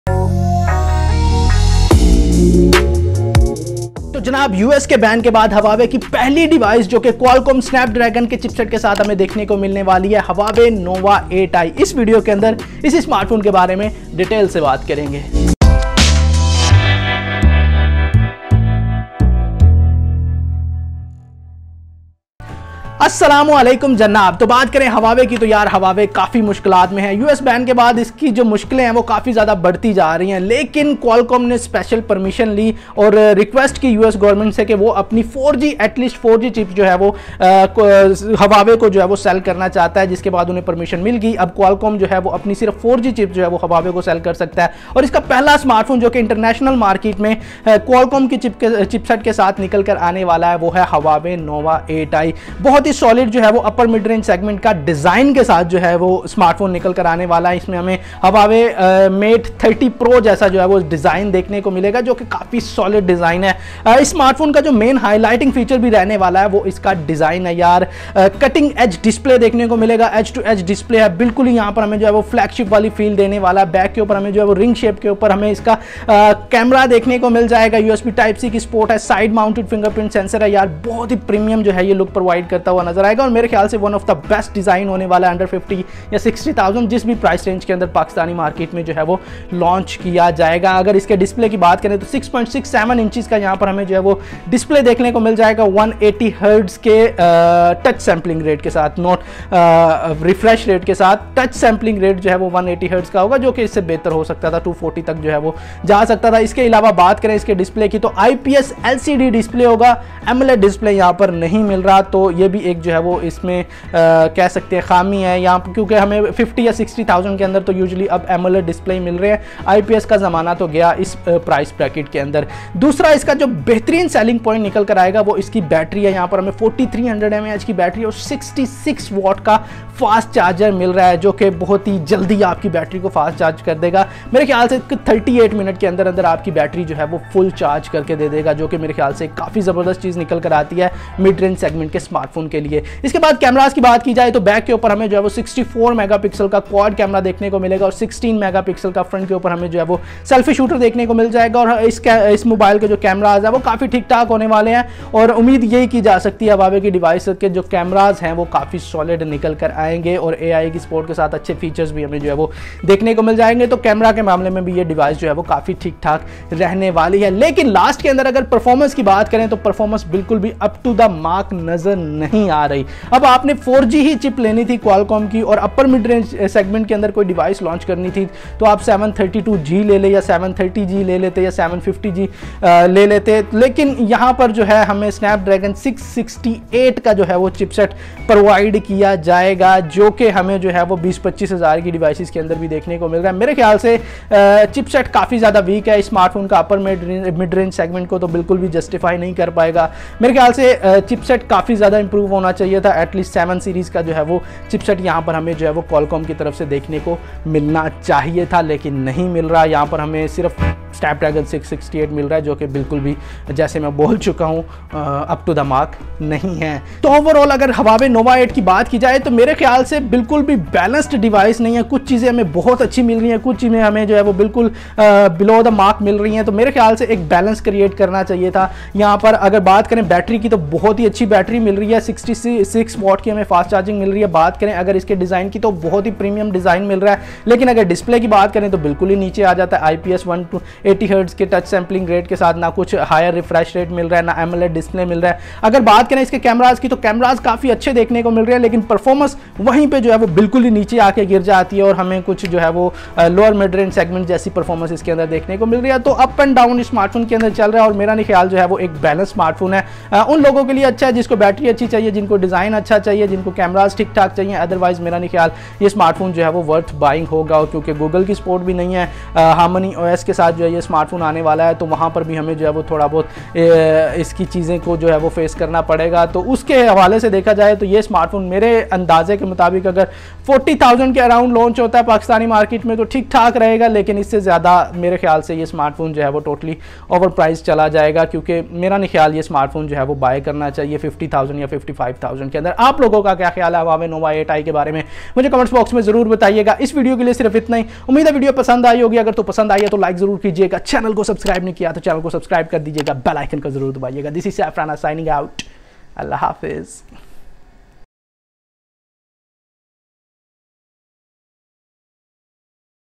Huawei Nova 8i Launched With 66W Charging and Premium Design
Huawei Nova 8i is Launched in Malaysia With 66W Charging and Premium Design lets look into it.

GuruRana is a Youtube Channel, where you will find technological News and videos, Keep yourself updated Everyday :)